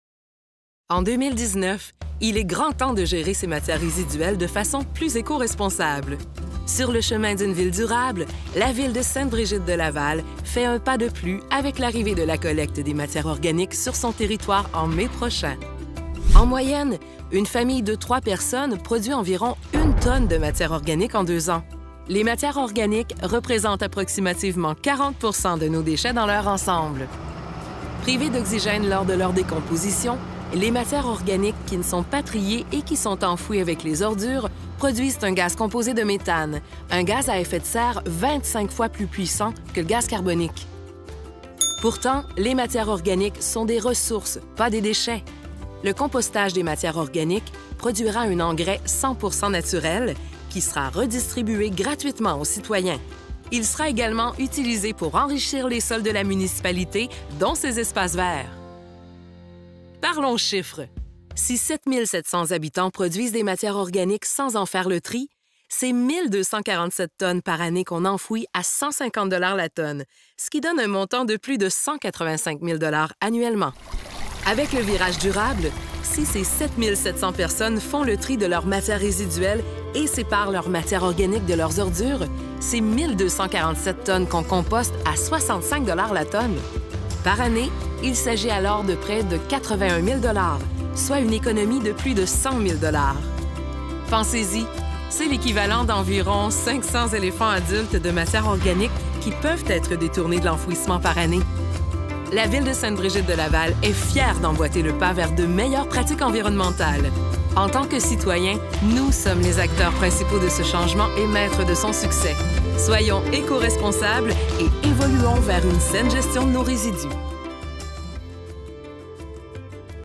Sainte-Brigitte-de-Laval fait un pas de plus vers une ville durable
En 2019, il est grand temps de gérer ses matières résiduelles de façon plus écoresponsable. C’est pourquoi la ville de Sainte-Brigitte-de-Laval fait un pas de plus vers une ville durable par l’arrivée de la collecte des matières organiques sur son territoire.